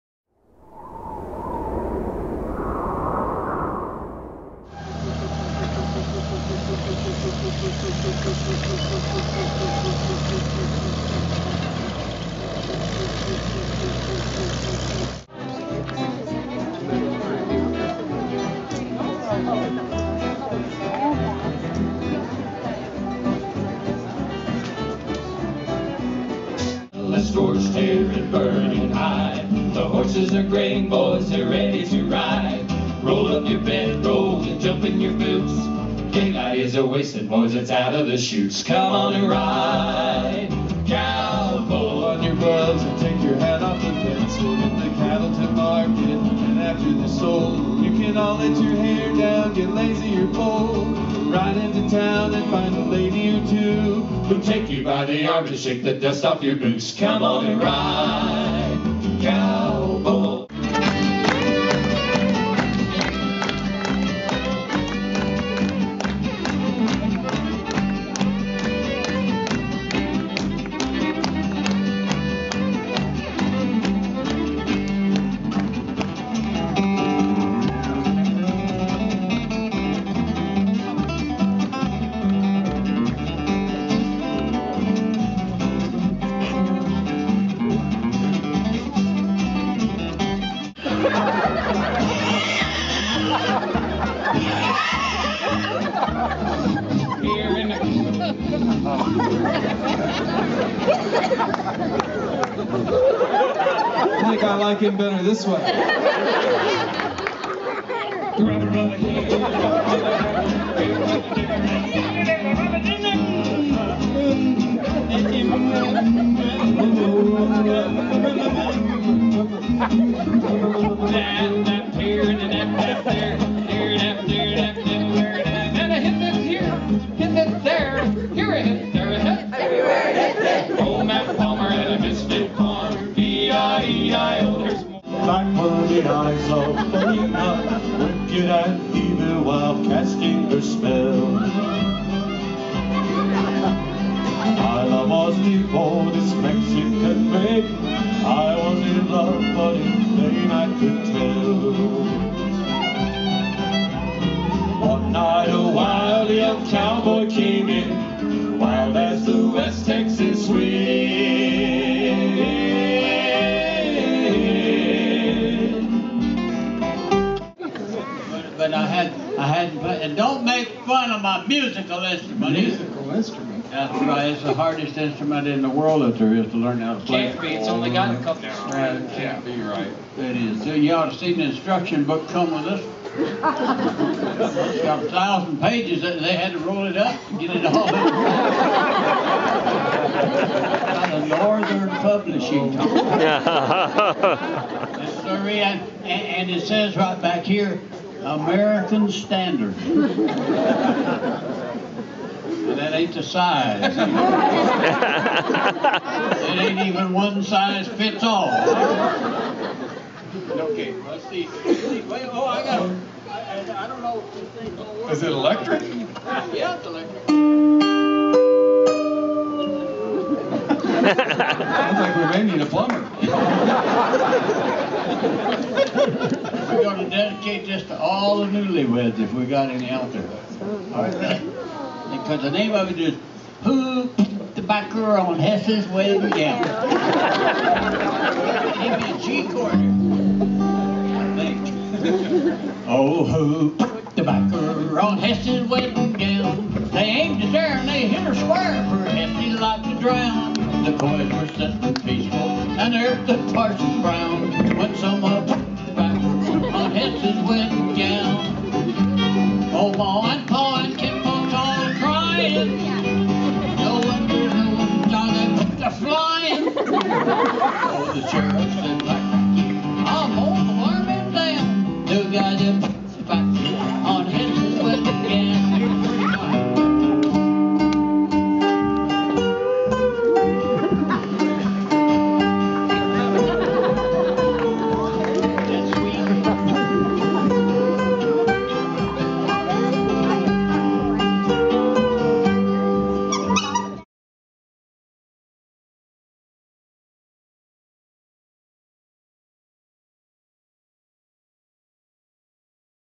Bar D Chuckwagon
Excerpts from the Bar D Chuckwagon Supper Show near Durango, Colorado